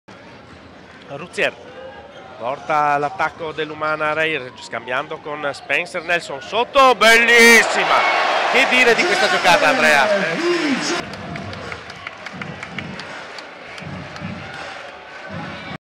Spettacolare alley-oop Nelson - Viggiano vs Cremona
Grande alzata di Spencer Nelson per Jeff Air Viggiano che chiude l'alley-oop facendo impazzire il Palasport Taliercio